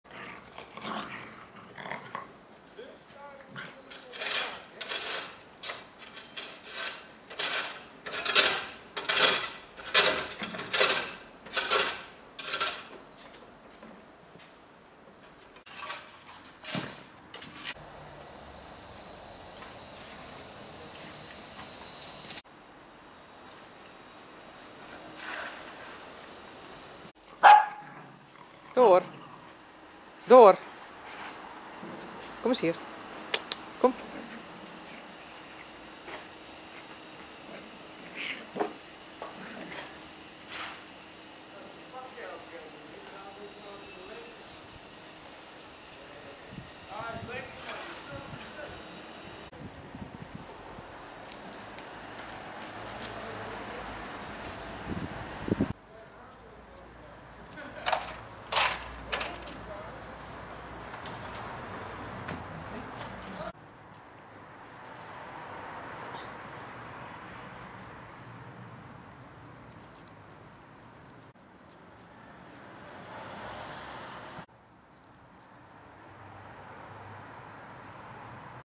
Tuinieren met pups
Beetje onkruid wieden, plantje bemesten, daarna nog wat snoeien. Inspectie van de druif. De tuin ligt er weer schitterend bij.